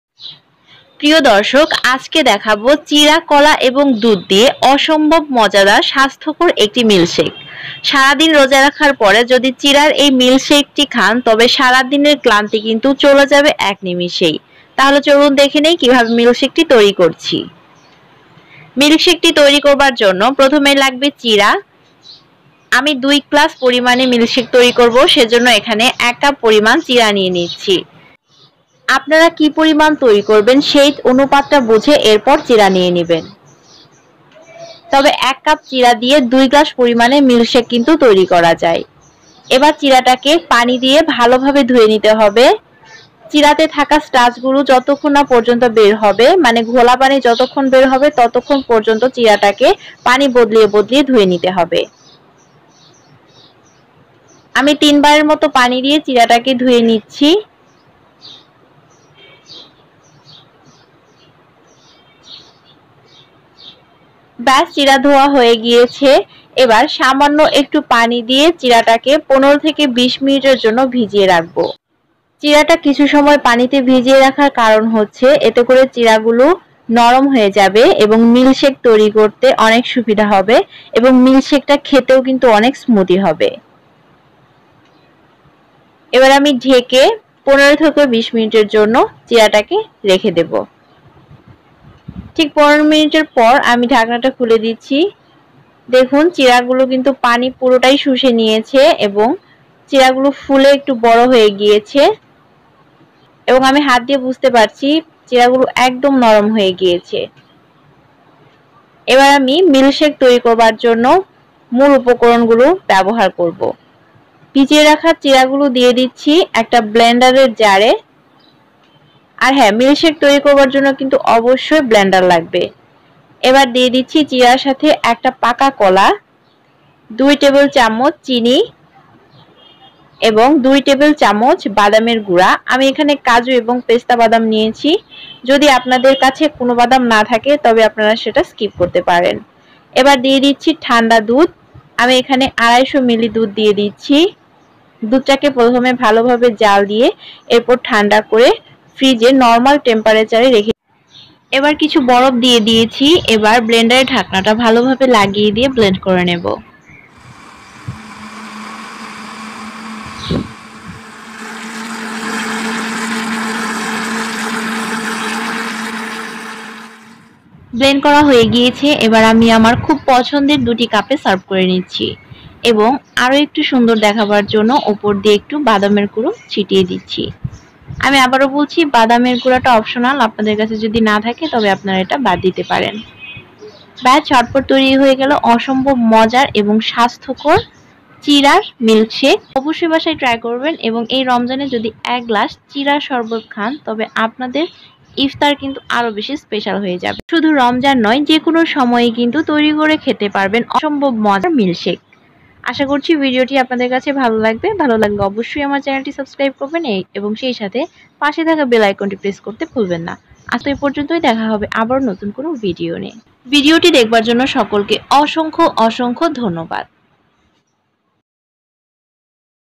চিড়া-কলার মজাদার লাচ্ছি শরবত || চিড়ার লাচ্চি -poha banana milkshake.
How to make poha banana milkshake 🍷🍷🍷🍷🍷

চিরার স্বাস্থ্যকর মিল্কসেক একবার খেলে মন জুরিয়ে যাবে🍺🍺🍺

চিরার মিল্কসেক এ আছে চিরা কলা দুধ ও বাদাম সব কিছু মিলিয়ে অসম্ভব মজার একটি মিল্কসেক।
বানাতে যা যা লাগবে ঃ
poha:1cup
Sugar-3tsp
Banana:1pcs
Kaju/pesta:5/6
Ice:5/6
if need water than 1cup cold water..
All ingredients mix well..
Thab ready testy poha milk shak..
chira_milkshak# banana_shak